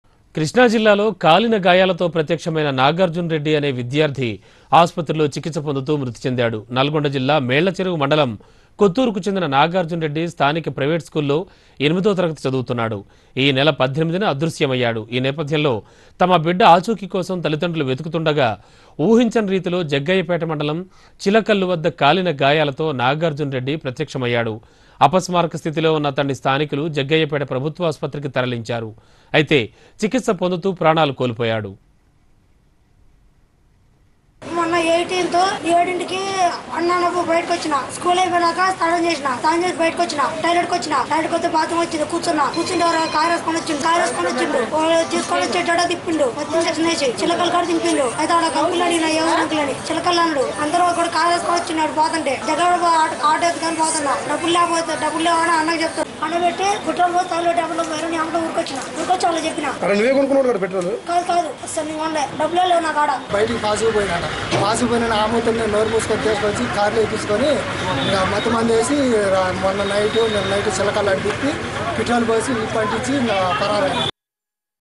Student found with injured body, dies in Hospital - Express TV
Student found with injured body, dies in Hospital